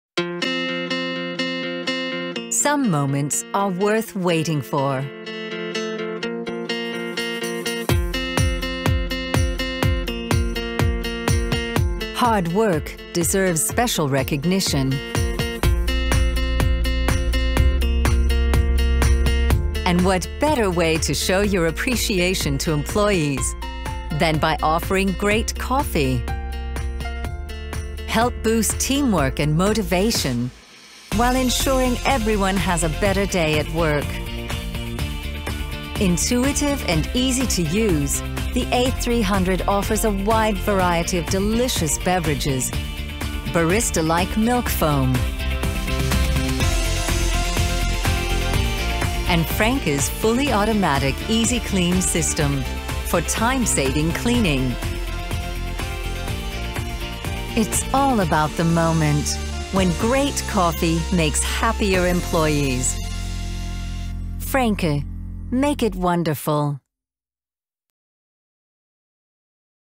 Franke A300 - Office Hero
It's all about the moment when great coffee makes happier employees. Whether for a departmental kitchen where space is limited or a large canteen, with our fully automatics your employees can expect high in-cup quality any time they need a break.